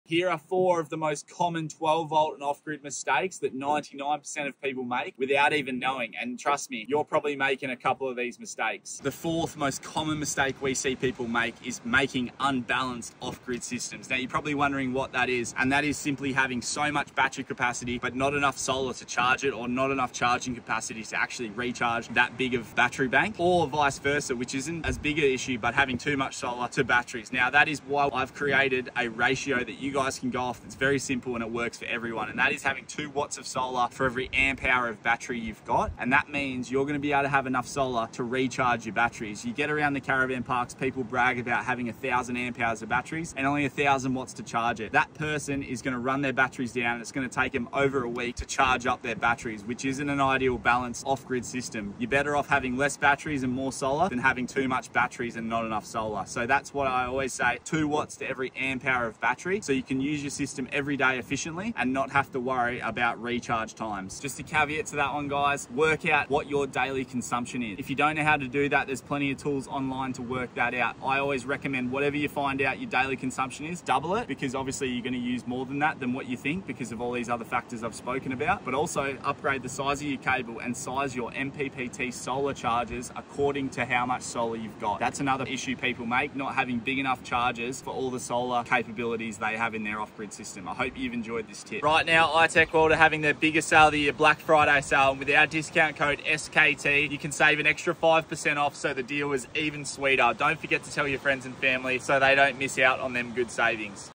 Too Much Battery, Not Enough Solar? Fix It Fast!
Too much battery, not enough solar? Here’s why that’s killing your setup 🔋We see it all the time - unbalanced systems that can’t keep up.Here’s our foolproof solar-to-battery ratio that works for 99% of people.⚙️ Watch and balance your system the right way!